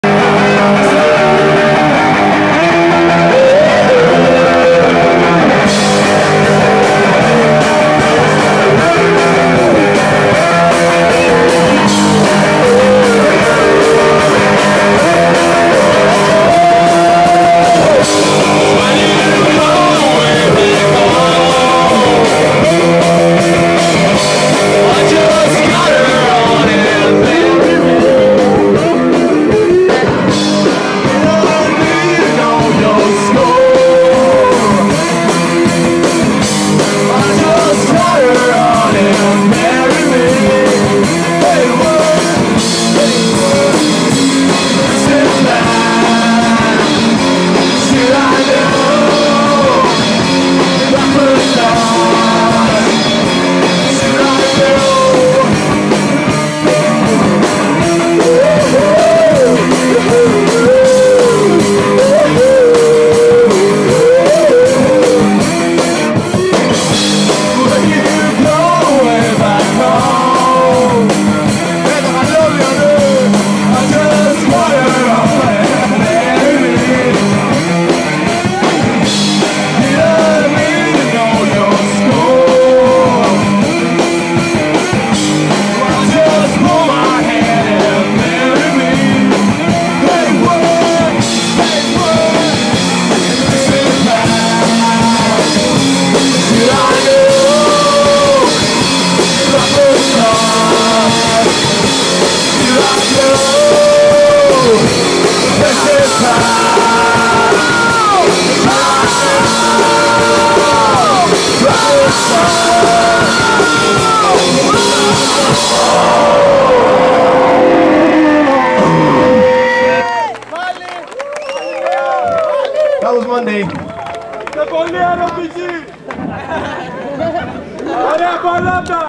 BAZOOKA-MONDAY-guerrillaaaaa in Volos
08-April-2010 Volos. Guerrilla gig with ACID BABY JESUS!